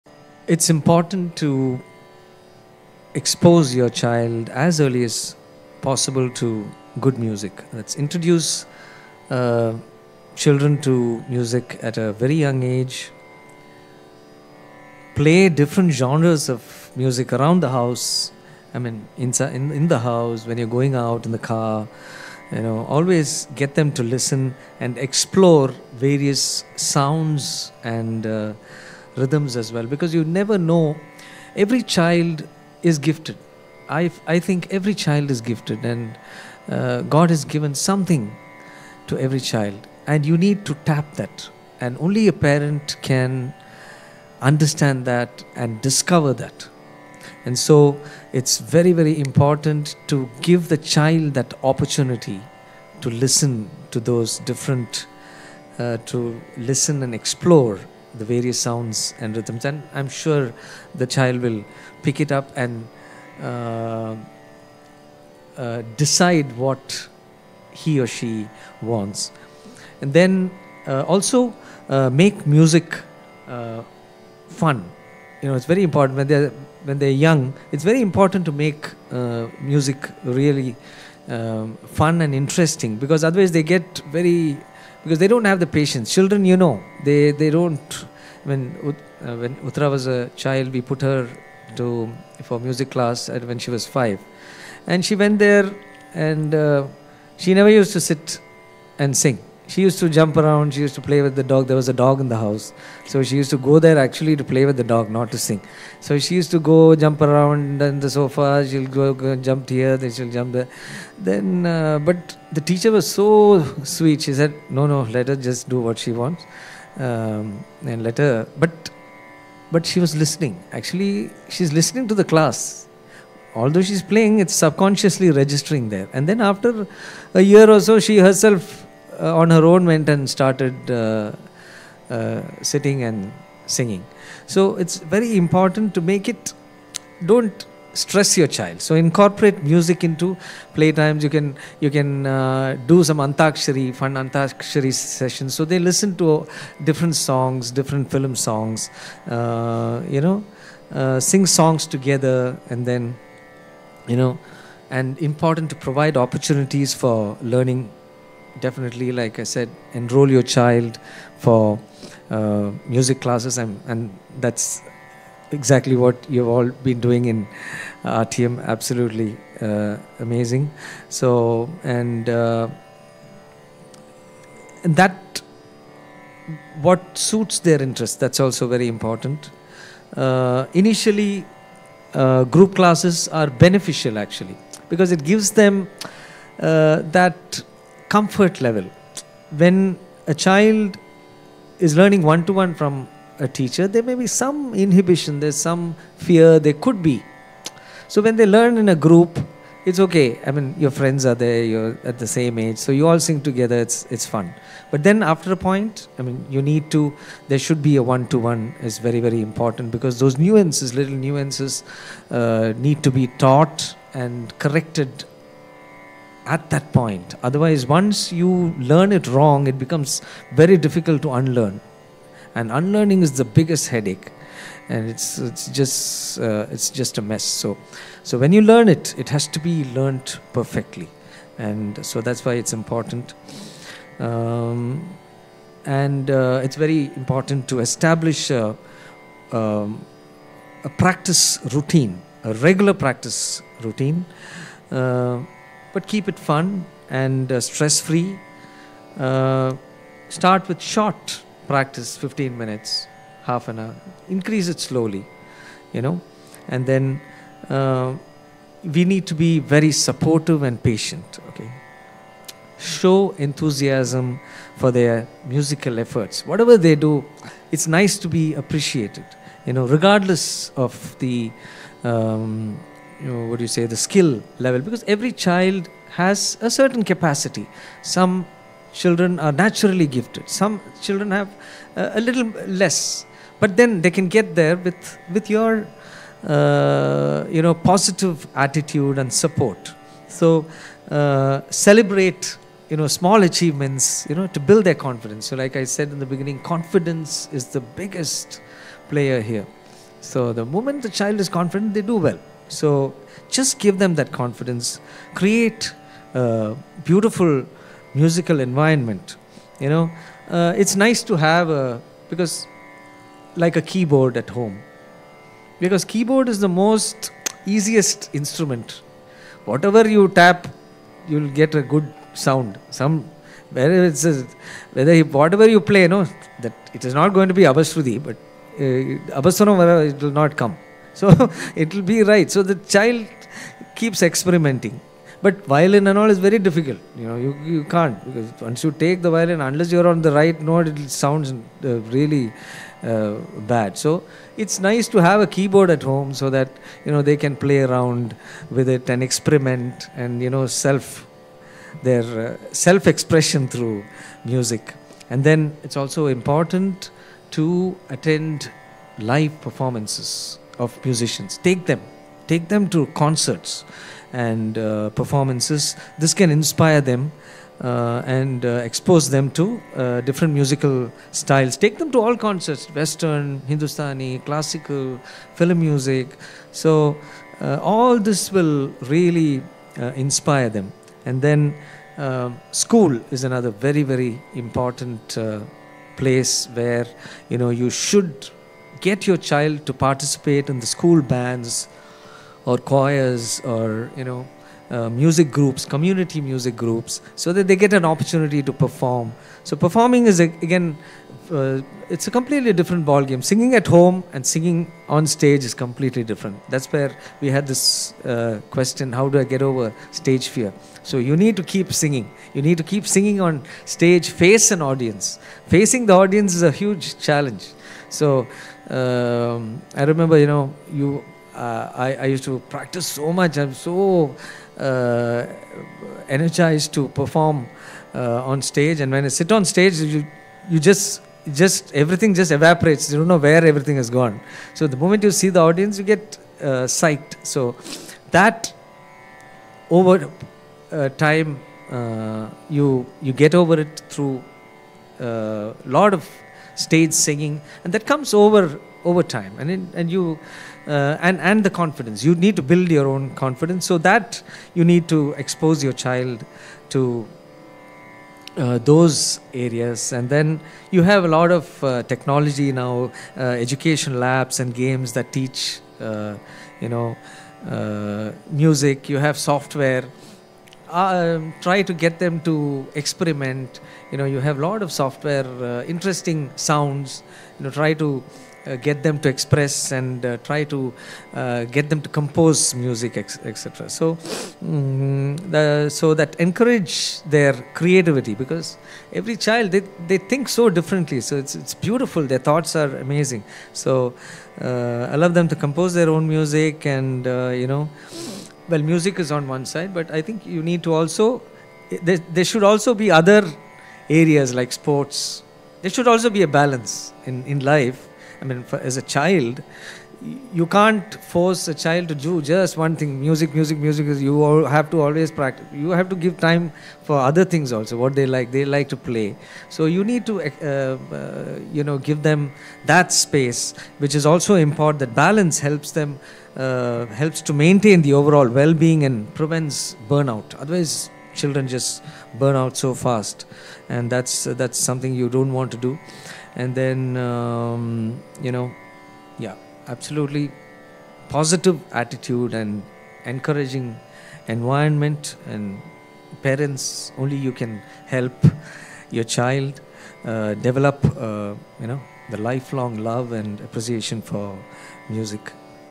Guiding Your Child in Music | Tips from P Unnikrishnan & Uthara Masterclass | Artium Academy
Join P Unnikrishnan and Uthara Unnikrishnan, the dynamic father-daughter duo, as they share their inspiring musical journey in this special Masterclass! In this engaging session, they offer valuable lessons, practical tips, and insightful feedback on navigating the world of music.

Watch live performances by Artium Academy's talented students as P Unnikrishnan and Uthara provide expert guidance and support. Whether you’re a parent looking to nurture your child's musical talent or a student eager to improve, this Masterclass is packed with wisdom you won’t want to miss!